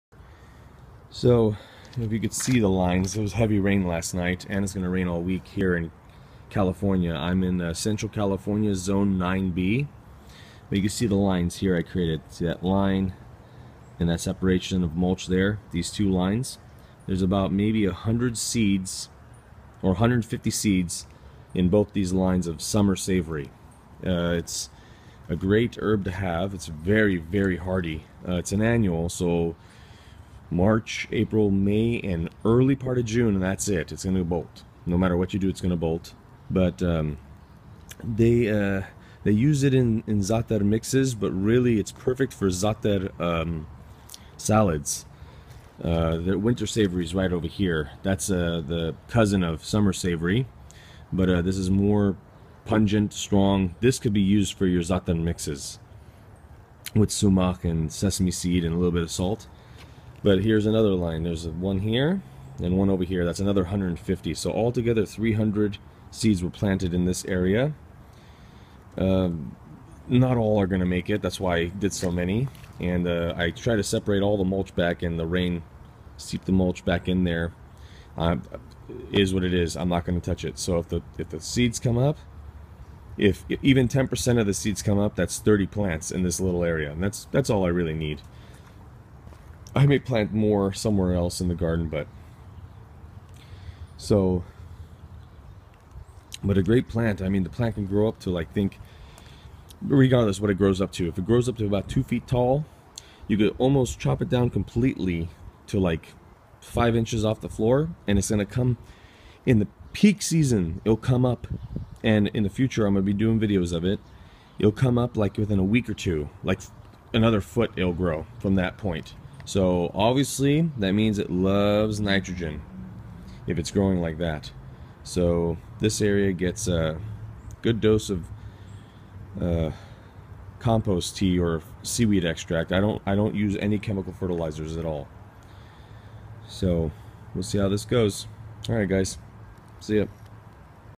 Planting Summer Savory Seeds - Zaatar - Satureja Hortensis
Planting Summer Savory Seeds - Satureja Hortensis; Great Hardy Annual Herb for Zaatar.